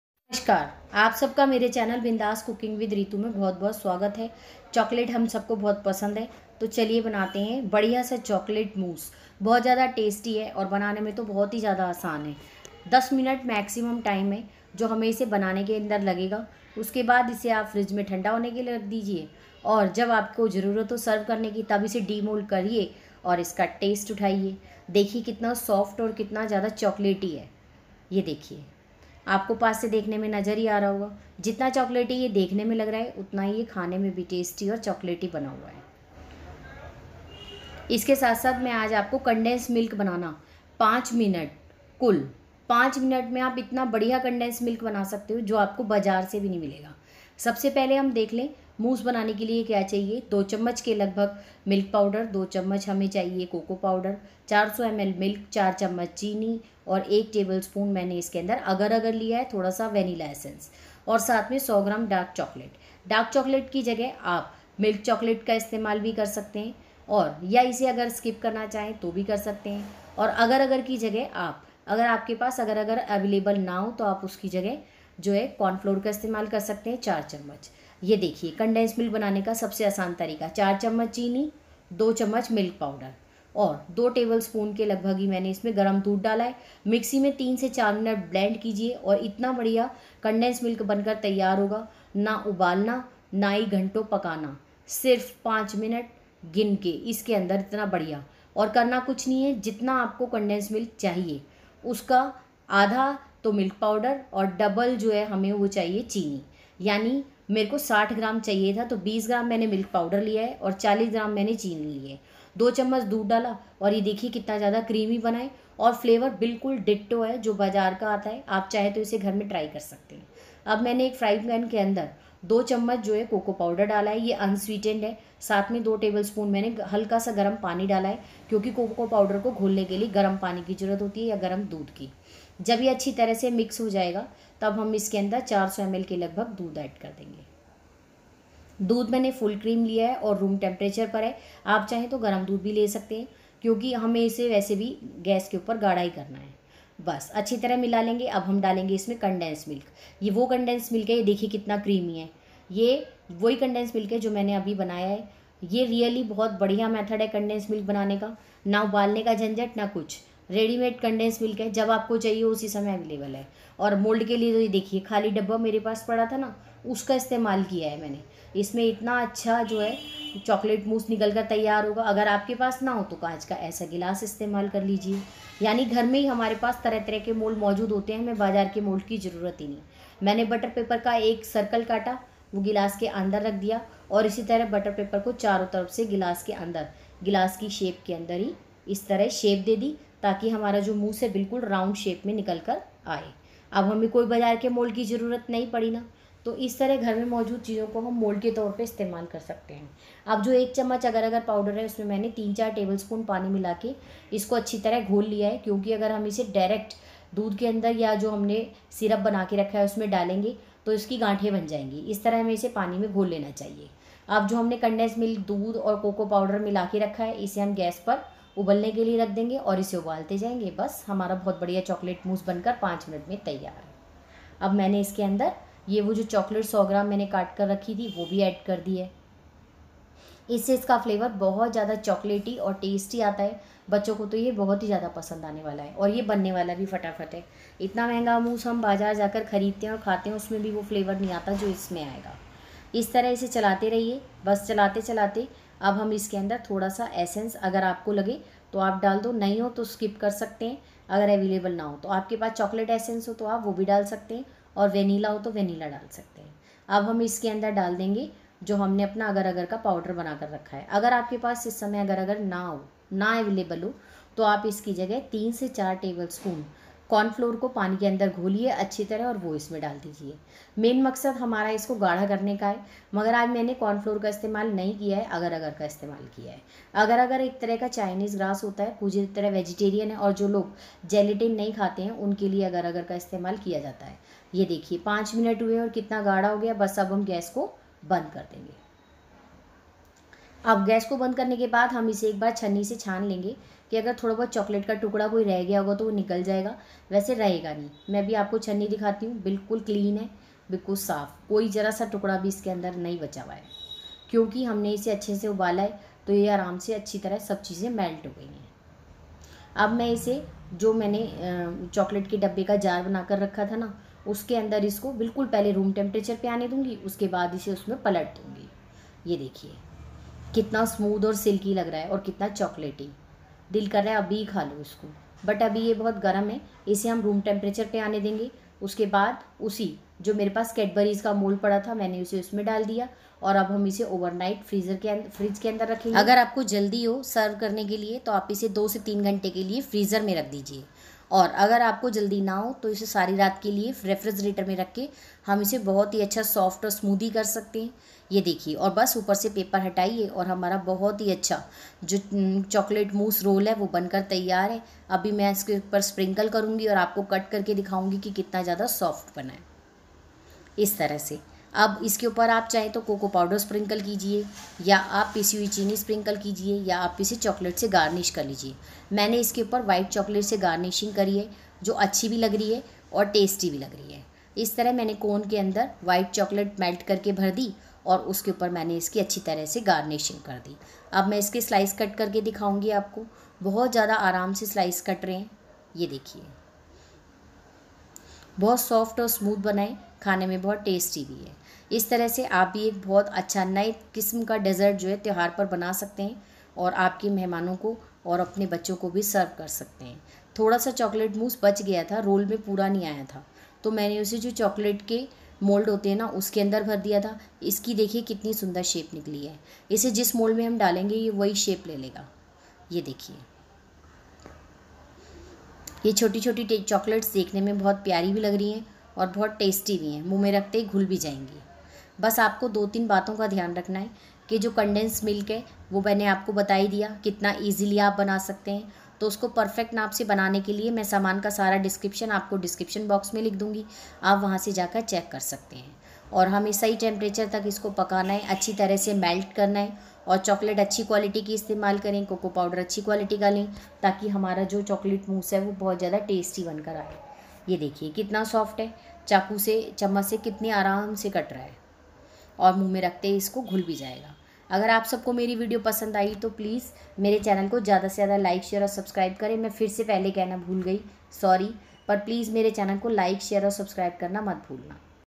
perfect chocolate mousse role cut cake||easy and new dessert for each occasion
ingredients:
milk-400ml
cocoa powder-2tbsp
dark chocolate-100gms
sugar-4tbsp
milk powder-2tbsp
vanilla/chcolate essence-1/4tsp
agar agar powder-1tbsp

tasty dessert, easy chocolate dessert,chcolate mousse role cake,how to make easy chcolate mousse cake